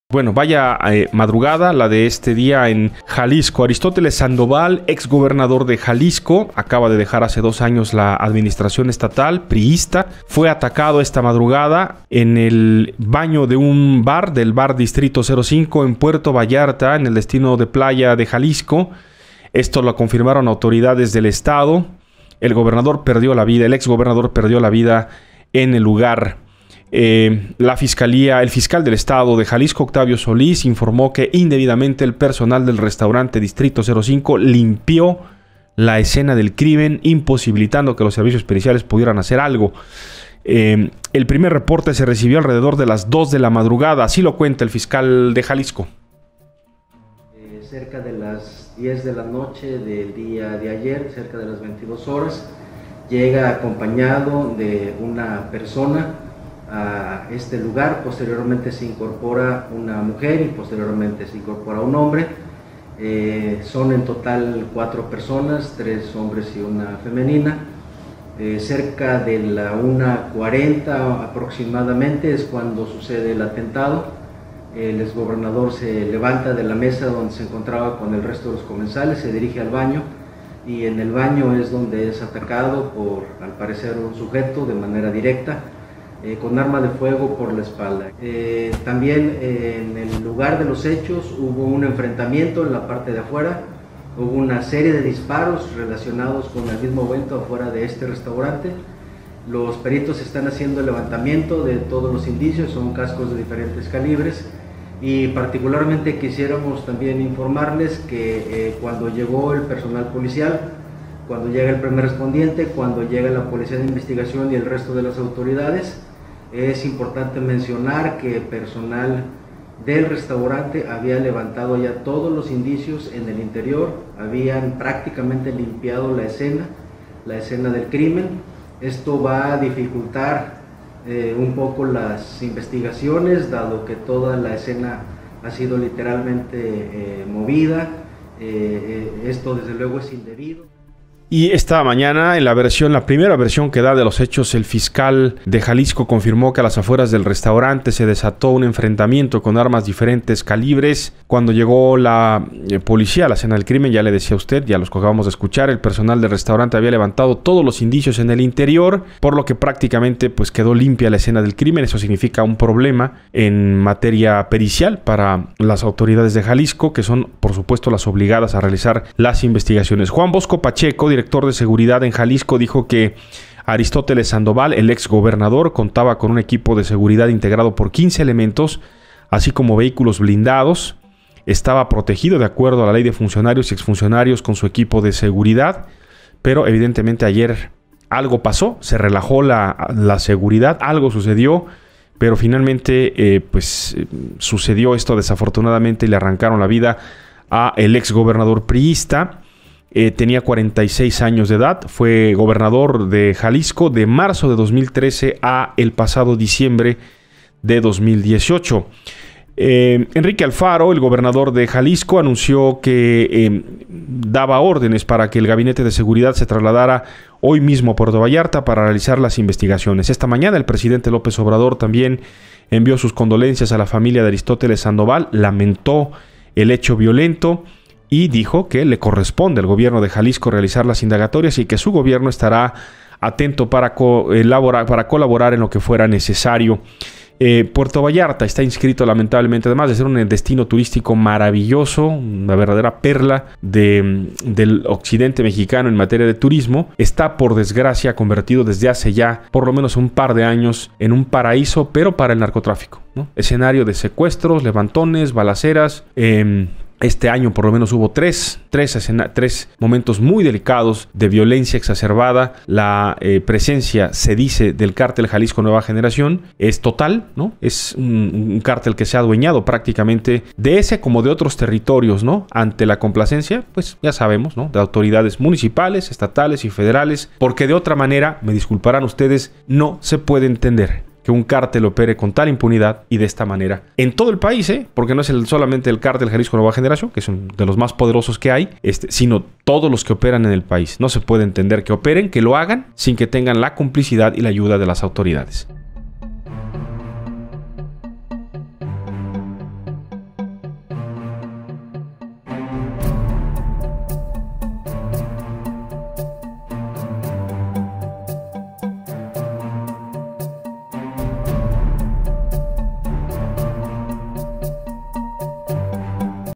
Al ex gobernador de Jalisco, Aristóteles Sandoval lo mataron en un bar en Puerto Vallarta
Aristóteles Sandoval, exgobernador de Jalisco, fue atacado a la 1:40 de la madrugada de este viernes, en el baño del bar distrito 05 en Puerto Vallarta.